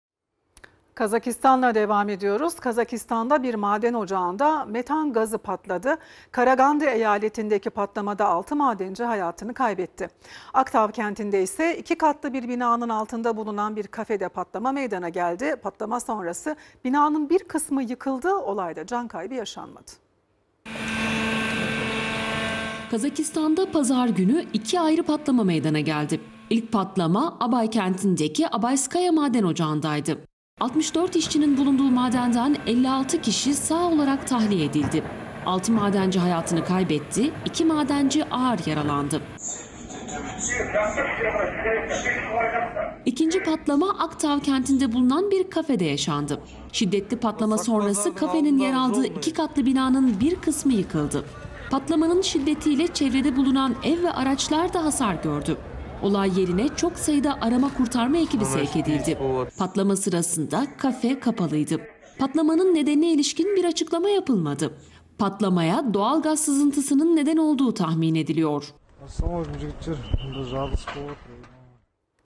Kazakistan'da İkinci Patlama-TRT AVAZ HABER-08.11.2021
Kazakistan’ın Aktau şehrinde iki katlı alışveriş binası patladı.
Etkili olan patlama anı kameralara yansıdı.
Yerel saatle akşam 21.20 sularında iki katlı binada bilinmeyen bir patlama meydana geldi.